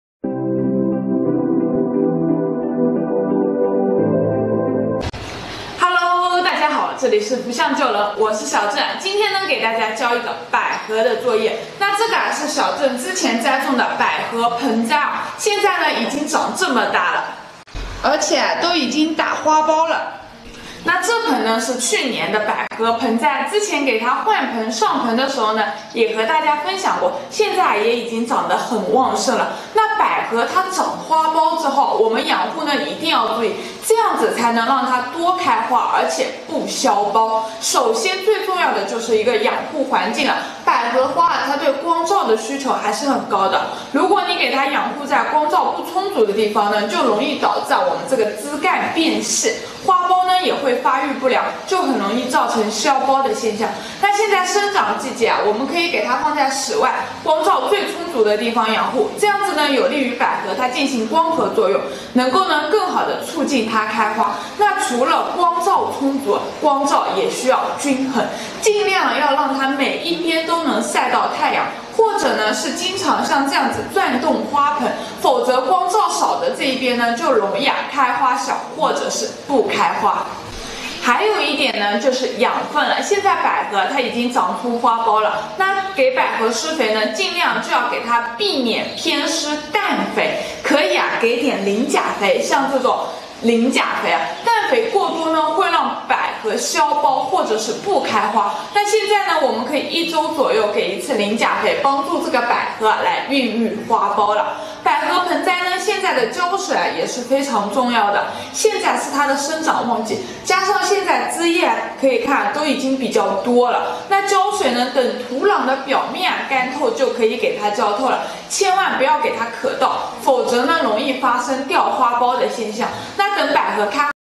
网购的百合种球长花苞了，养护注意这几点，不倒伏，开花又大又香
网购的百合种球长花苞了，养护注意这几点，不倒伏，开花又大又香,本视频由浮巷旧人聊花卉原创提供,2234次播放,好看视频是由百度团队打造的集内涵和颜值于一身的专业短视频聚合平台